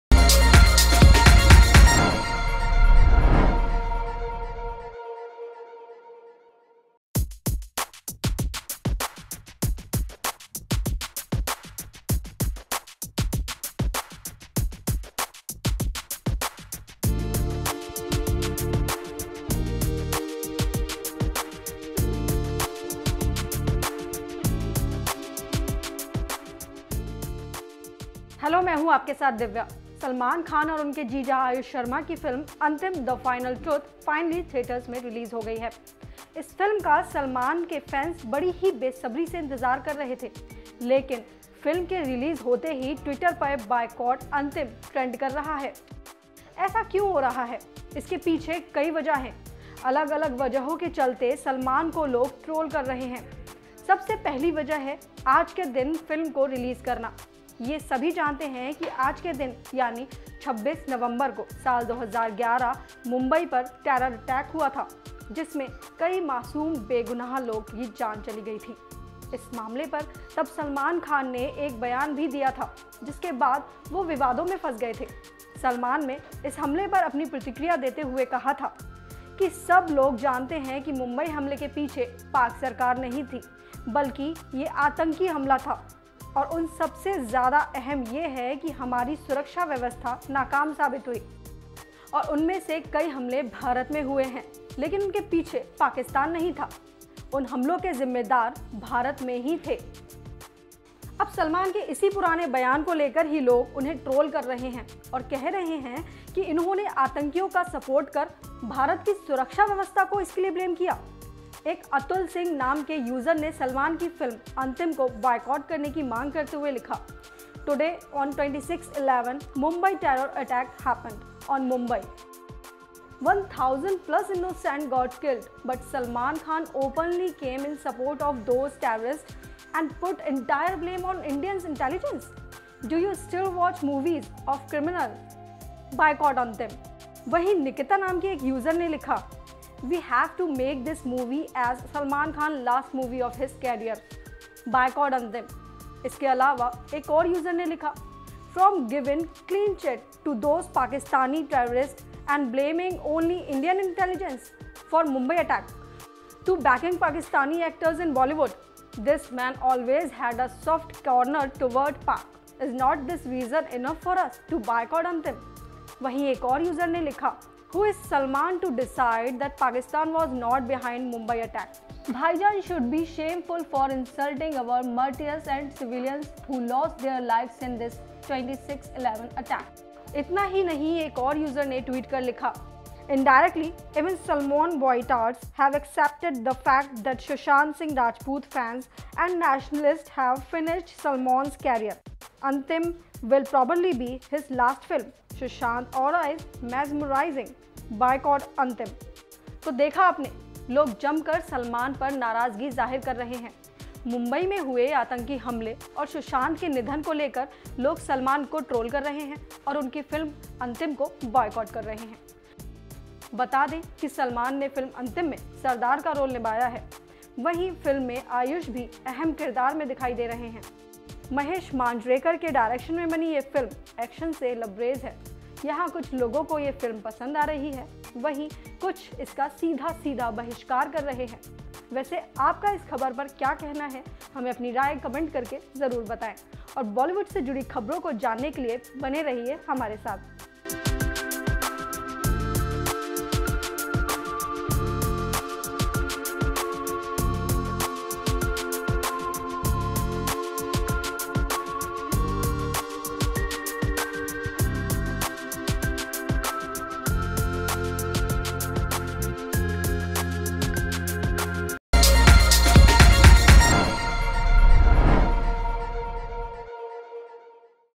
Salman Khan की फिल्म Antim पर भड़के लोग, Boycott करने की दी धमकी
सलमान खान और उनके जीजा आयुष शर्मा की फ़िल्म antim the final truth फाइनली थियेटर्स में रिलीज हो गई है. इस फिल्म का सलमान के फैंस बड़ी ही बेसब्ररी से इंतजार कर रहे थे,
thumb02: Twitter पर क्यों Trend हुआ Boycott Antim
Chandrakala
prashant.